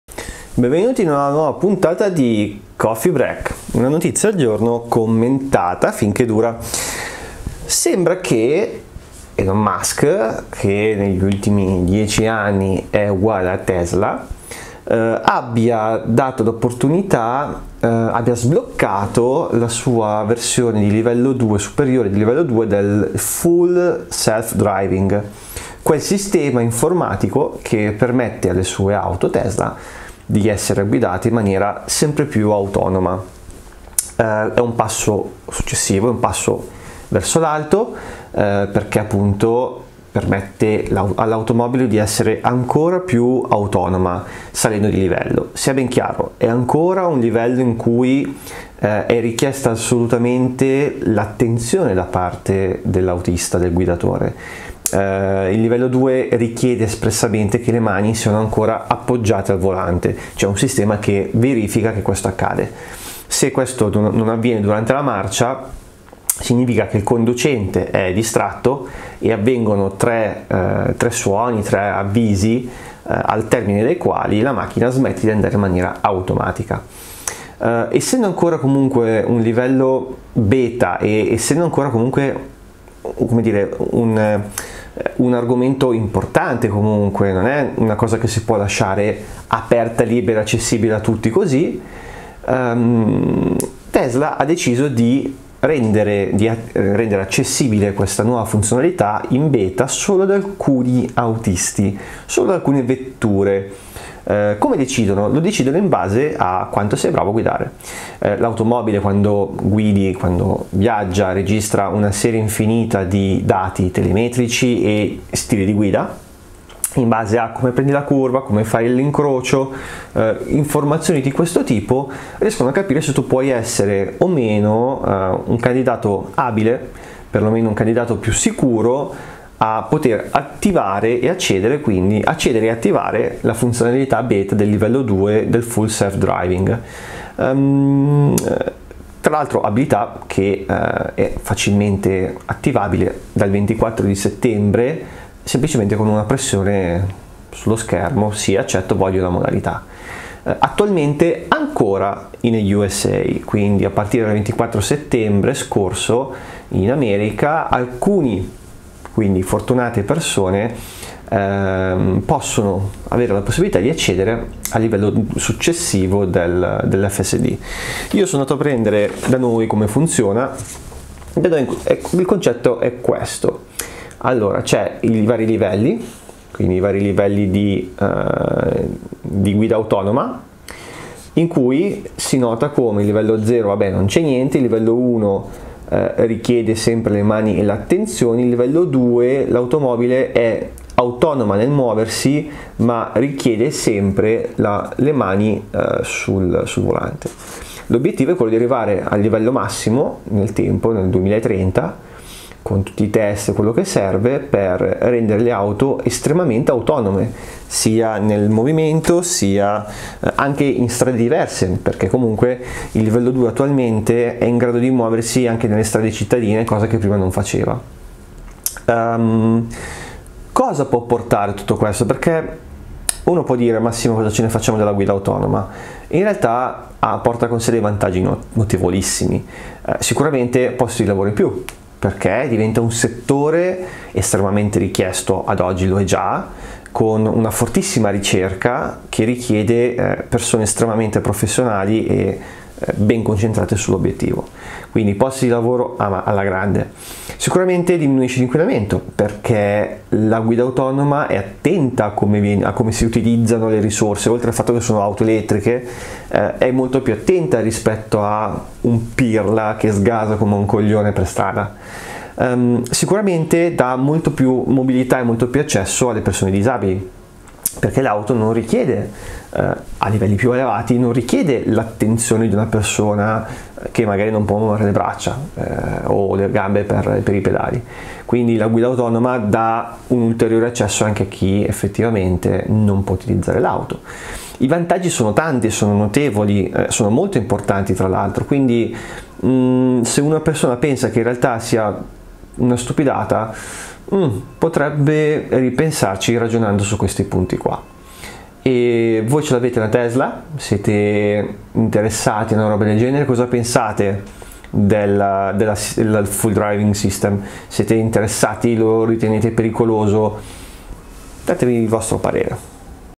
Il Full Self Driving porta dei benefici a tutti?
Tesla ha reso disponibile sulle sue vetture il Full Self Driving di livello 2, ma solo se i dati sulla tua guida dimostrano che sei un autista attento. É una buona idea, se si pensa che questo livello è ancora in beta (versione 10.0.1) che permetterà di rendere questo livello aperto a tutti, con dei test veritieri e sicuri. Questo perchè il livello 2 del FSD, richiede ancora che l'autista sia attento alla strada tenendo le mani sul volante e se si distrae viene avvisato con 3 allarmi, dopo di chè il sistema automatico si spegne obbligandoti a ritornare alla modalità manuale.

Ma tutto questo, porta dei benefici anche a noi "comuni mortali"? Che cosa se ne fa la società della guida totalmente autonoma? I vantaggi sono diversi, dai posti di lavoro estremamente specializzati che si creano, al minor inquinamento per poi dare inoltre a un disabile, la possibilità di muoversi in auto anche se non può guidare.